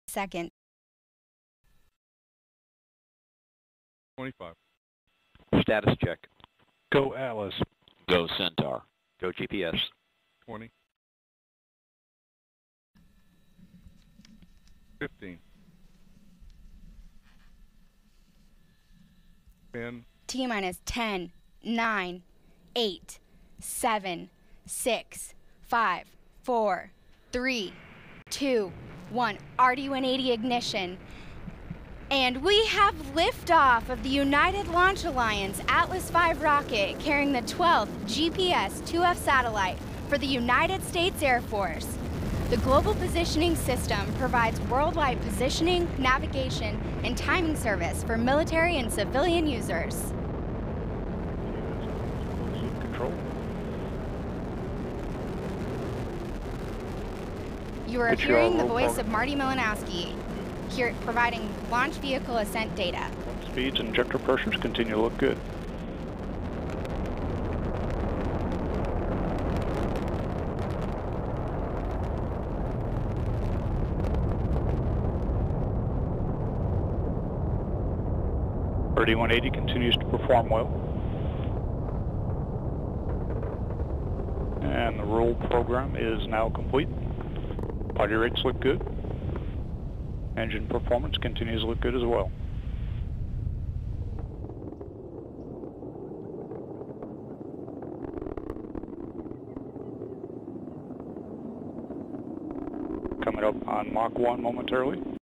Launch of ULA Launch Atlas-V 401 configuration carrying GPSII-F12
Launch of ULA Launch Atlas-V 401 configuration carrying GPSII-F12 from CCAFS Feb. 5 at 8:38 am EST